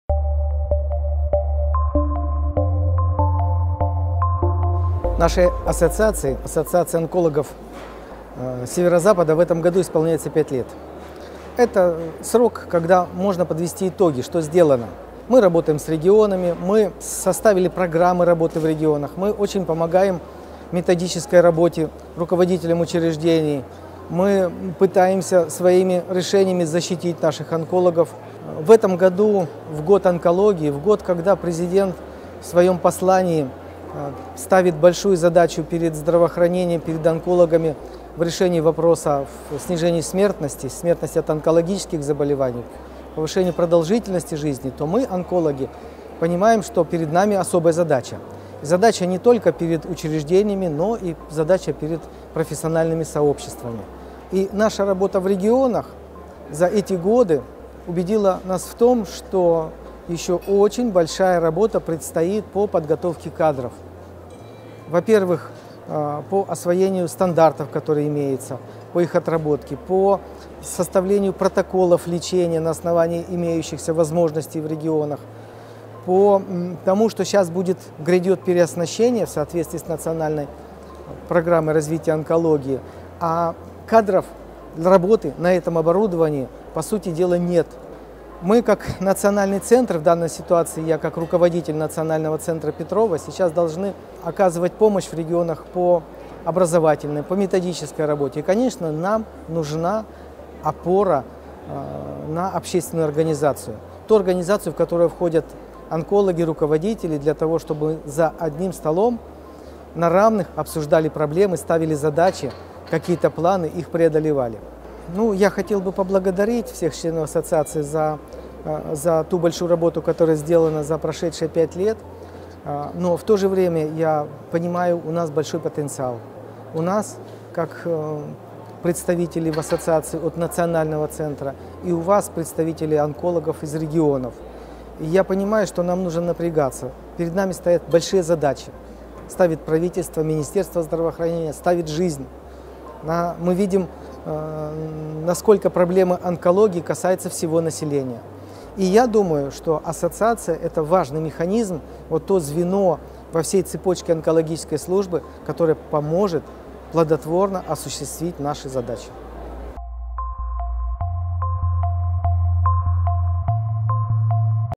О Ассоциации онкологов СЗФО А.М. Беляев
Беляев Алексей Михайлович
Президент Ассоциации онкологов СЗФО, Директор ФГБУ "НМИЦ онкологии им. Н.Н. Петрова" Минздрава России